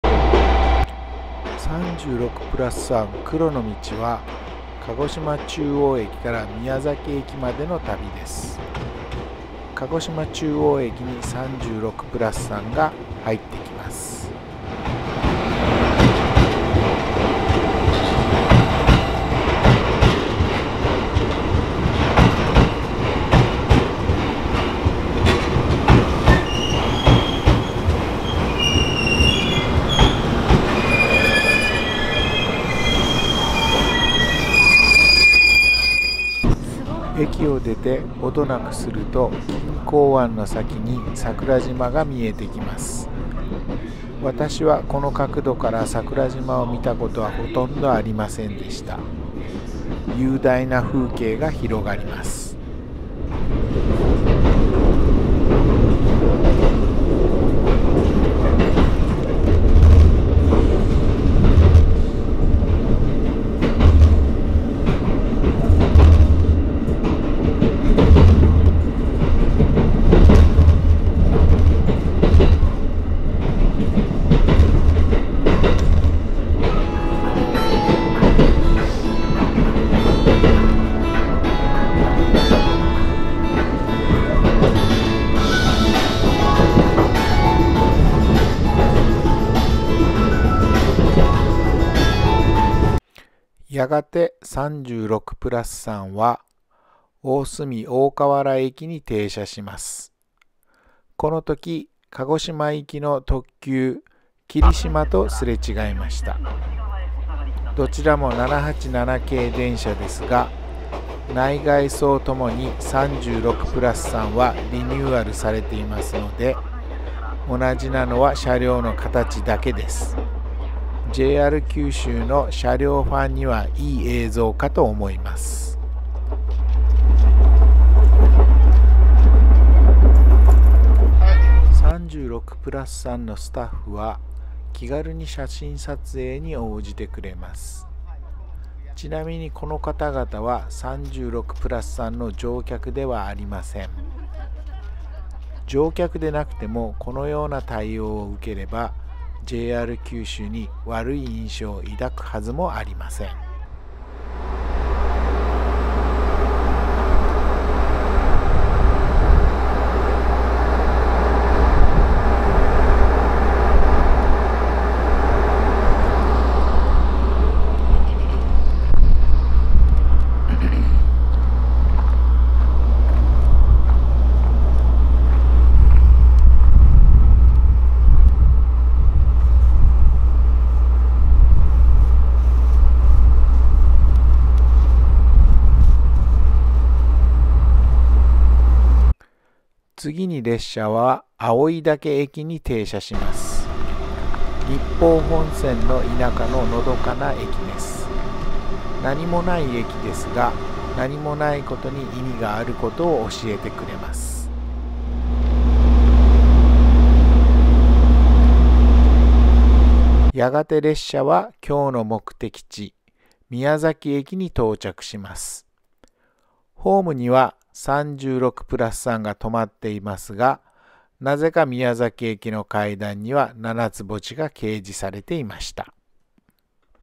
【36ぷらす3】黒の路（鹿児島中央駅-宮崎駅)
「36ぷらす3」　黒の路「鹿児島中央駅」から「宮崎駅」への旅